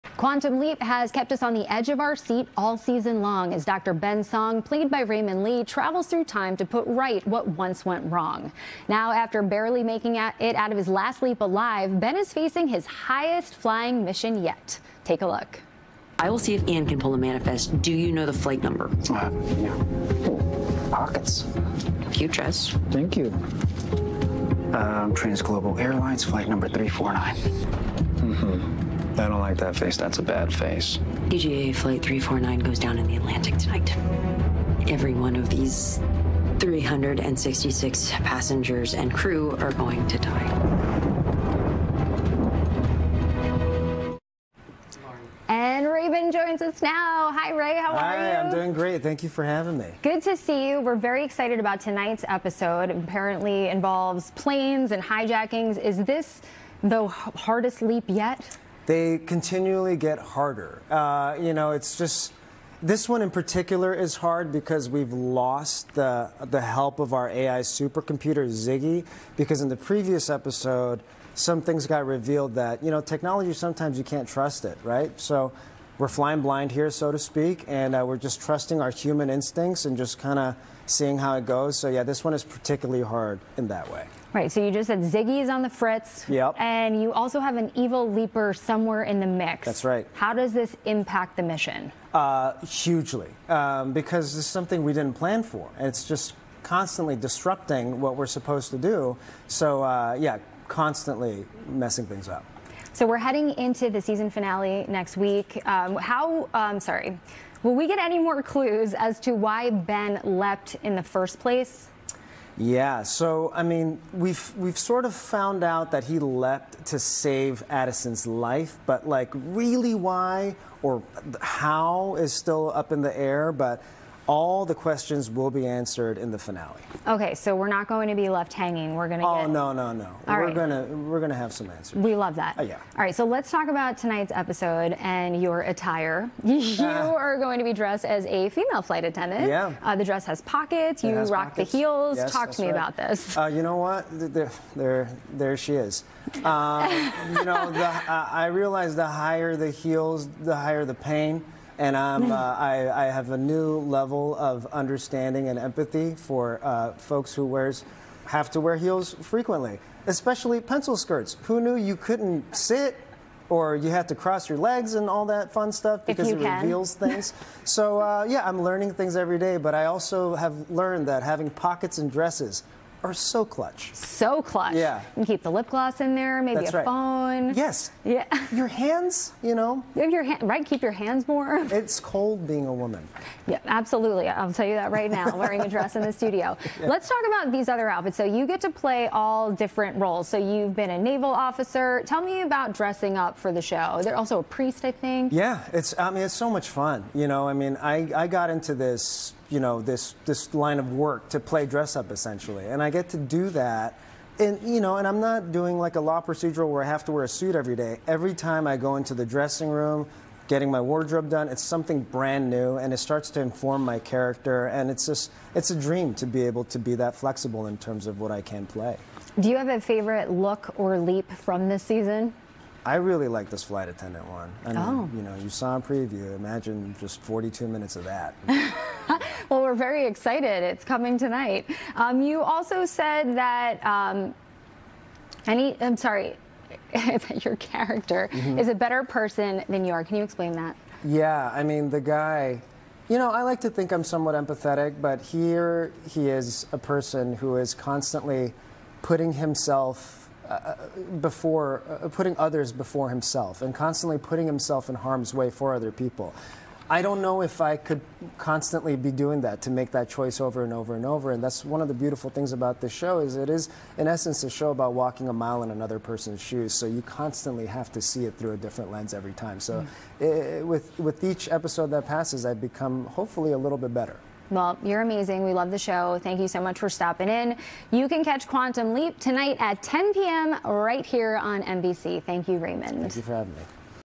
Raymond Lee Reveals Favorite Look Of "Quantum Leap" | New York Live TV
Raymond Lee previews the exciting end of the first season of "Quantum Leap" with Lauren Scala.